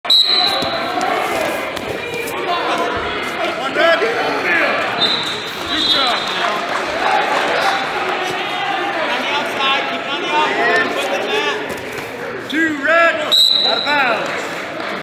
How to escape in one easy step
December 29, 2021 — Don Flavin Dual Team Invitational Tournament at DeKalb High School in DeKalb, Illinois

138 – Trent DuMont {G} of Sycamore IL vs. Colin Roque {R} of Chicago Taft IL